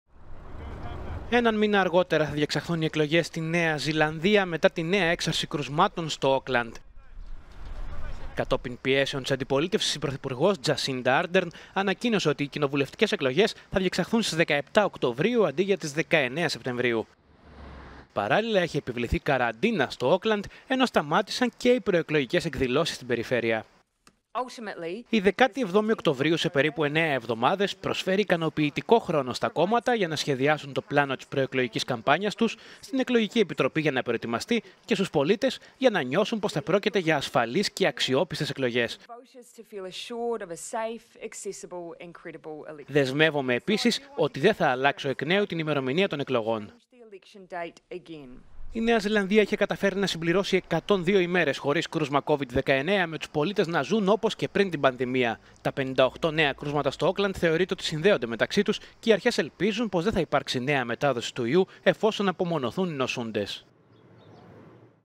Μετάθεση των εκλογών στη Ν.Ζηλανδία λόγω COVID-19
Στις 17 Οκτωβρίου θα διεξαθούν οι γενικές εκλογές αντί για τις 19 Σεπτεμβρίου